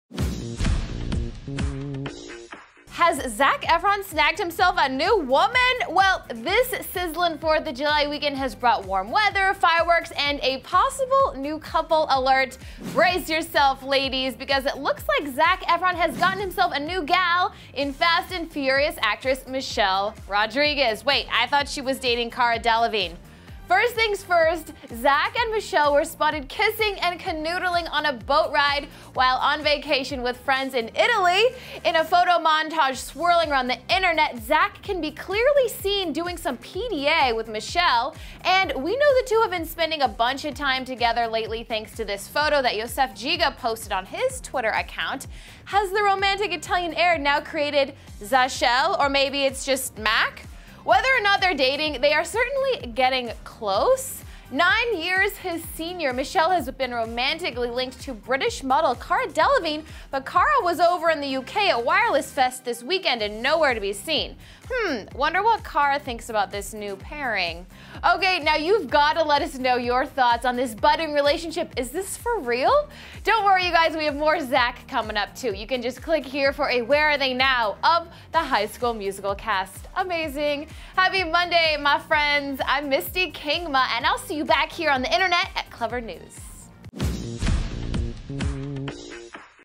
Zac Efron Spotted Kissing Michelle Rodriguez! New Couple Alert?
Has Zac Efron snagged himself a new woman? Well, this sizzling 4th of July weekend has brought warm weather, fireworks, and... a possible new couple alert! Brace yourself, ladies, because it looks like Zac Efron has got himself a new gal in Fast & Furious actress, Michelle Rodriguez! Wait- I thought she was dating Cara Delevingne? First things first: Zac and Michelle were spotted kissing and canoodling on a boat ride while on vacation with friends in Italy. In this photo montage swirling around the internet, you can clearly see the not-so-secretive PDA between Zac and Michelle. And we know the two have been spending a bunch of time together lately thanks to this photo that Youssef Giga posted on his Twitter account. Has the romantic Italian air now created Za-chelle (or maybe it's Mac?) Whether or not they're dating, they are certainly "getting close". Nine years his senior, Michelle has also been romantically linked to British model Cara Delevingne, but Cara was over in the UK at Wireless fest this weekend. Hmm, we wonder what Cara thinks about this new pairing? Ok, now you've got to let us know your thoughts on his budding relationship, is this for real? Don't worry more Zac is coming... click here for a "where are they now" of the High School Musical cast! Happy Monday guys, I'm Misty Kingma and I'll see you back here on the Internet on ClevverNews.